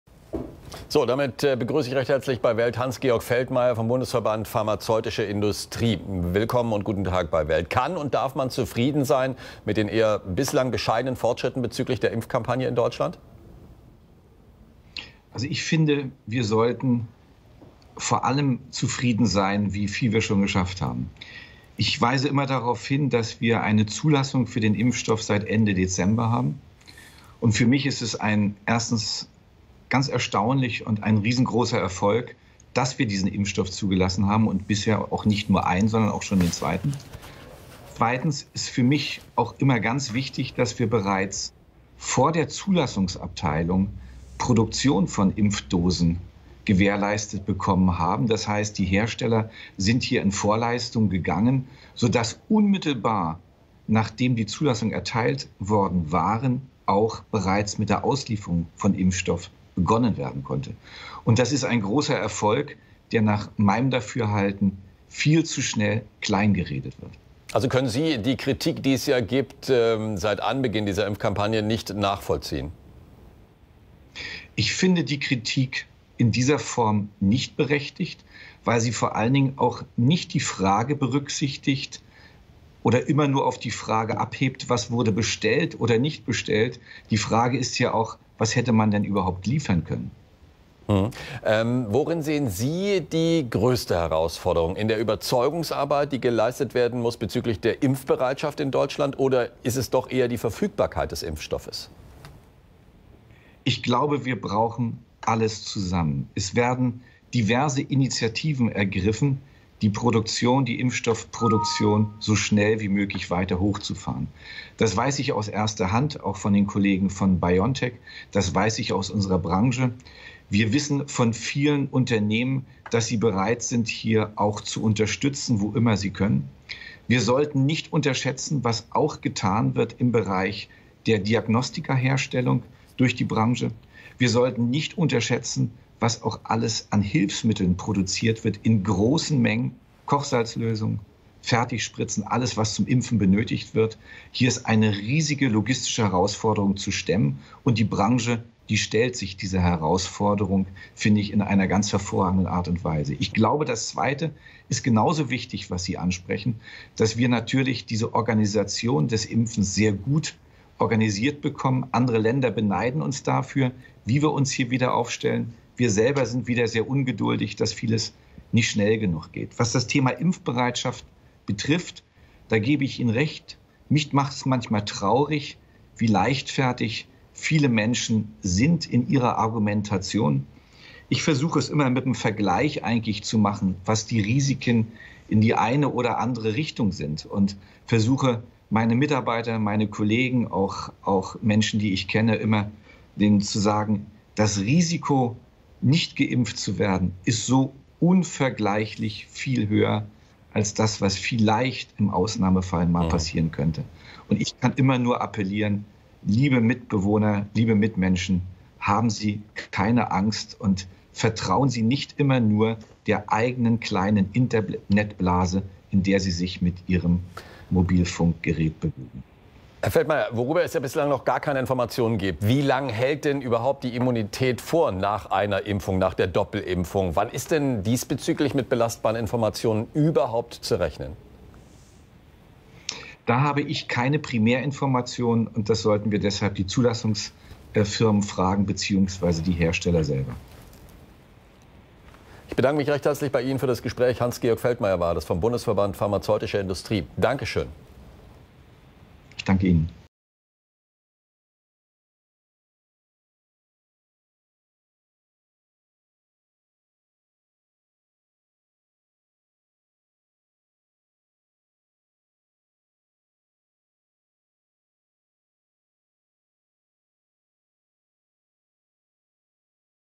CORONA-IMPFUNGEN: "Wir sollten vor allem zufrieden sein, wieviel wir schon geschafft haben"
Hans-Georg Feldmeier vom Bundesverband Pharmazeutische Industrie, zu den Fortschritten der Impfungen in Deutschland und zu der Herausforderung, die Impfbereitschaft der Bevölkerung zu steigern.